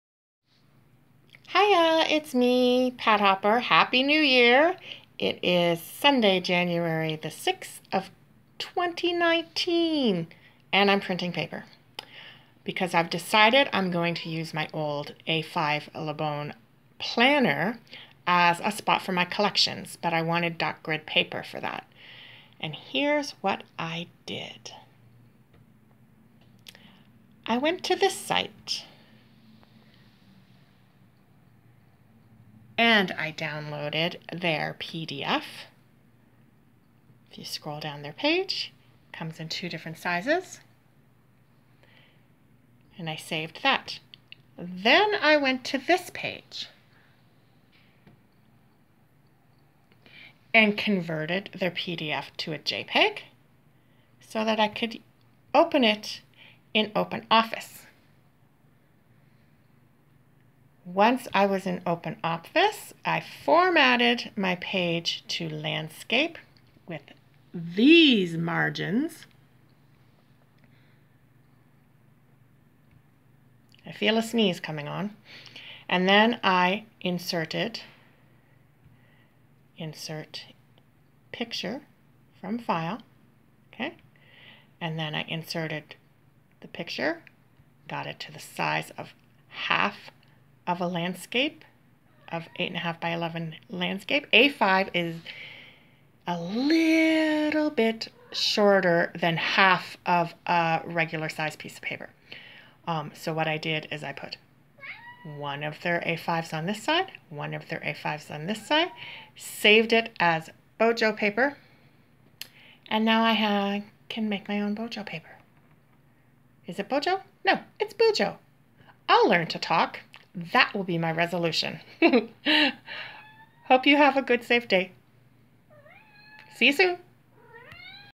Making dot grid paper for my Bullet Journal Collections
A quick cell phone video to explain how I made dot grid paper for my bulletjournal

Free downloadable Dot grid template here

I didn't really like how the PDF printed. I am hoping to get 2 a5 sheets on 1 landscape sized document so that I don't have to cut the paper.

So ...

Convert a pdf to a jpeg here

I used downloaded the a5 size and just reshaped a them a bit, so 2 could fit side by side in a landscape Open Office text document.

Apache Open office is a free program that can be found here

When your paper is printed out, you will be able to cut the page down the vertical center and get 2 sheets of A5 size dot grid paper for your homemade

Hope these links and this quick video help you.

Ps the meowing kitten in the background is why I haven't been uploading all that regularly. She demands to be the center of attention and of not she will meow and howl at the top of her lungs until that changes.  Annoying af really.